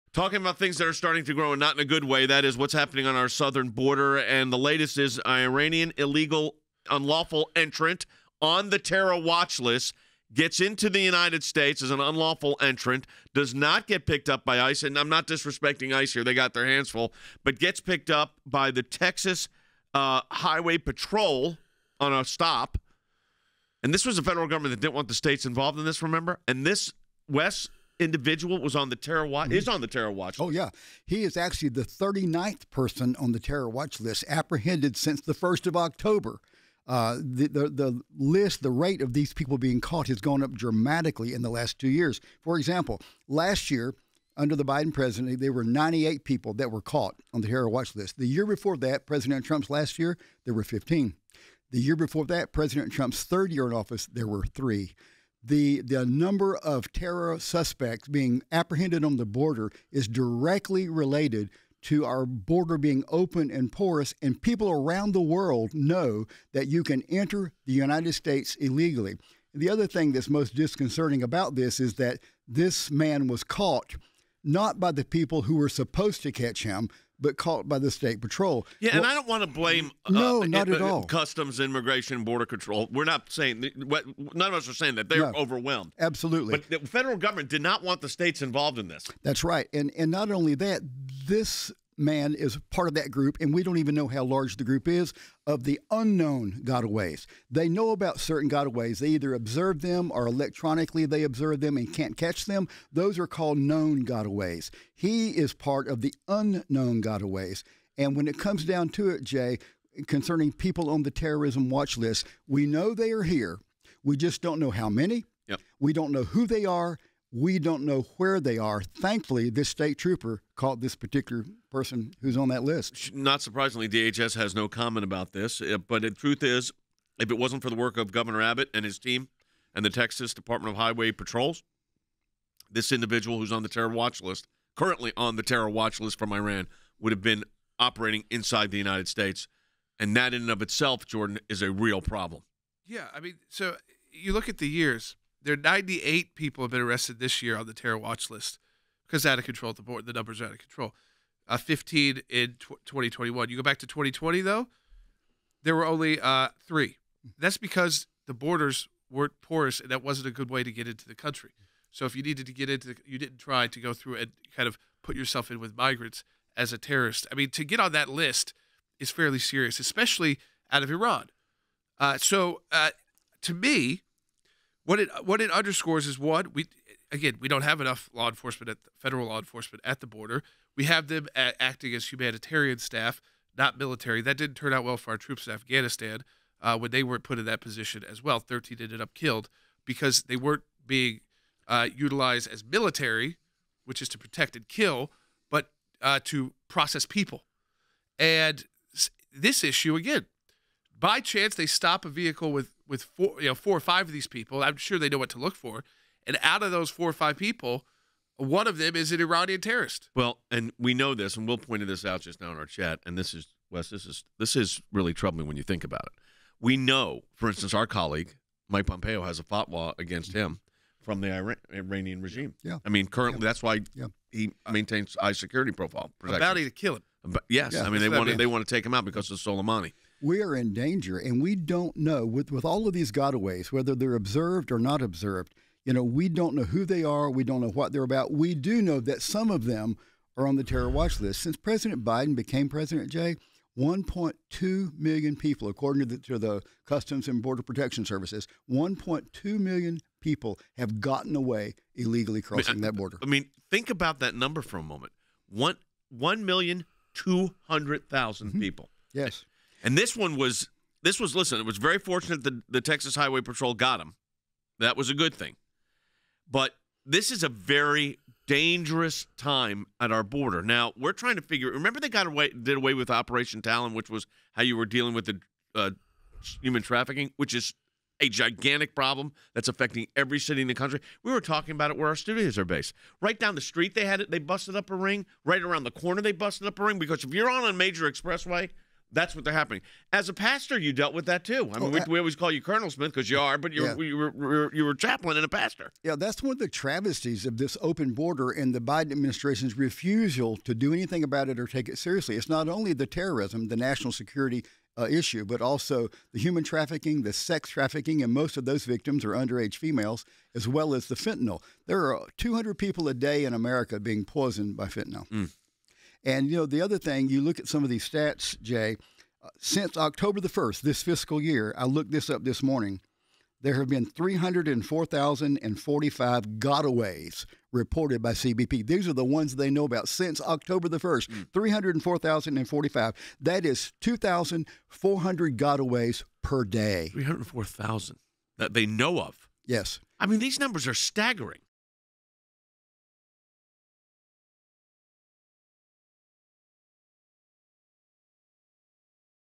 Iranian Terrorist Caught at Southern Border
An Iranian terrorist, who is currently on the Terrorist Watch List, was recently apprehended by Texas Highway Patrol. This is a direct result of Biden’s failure to secure the border properly.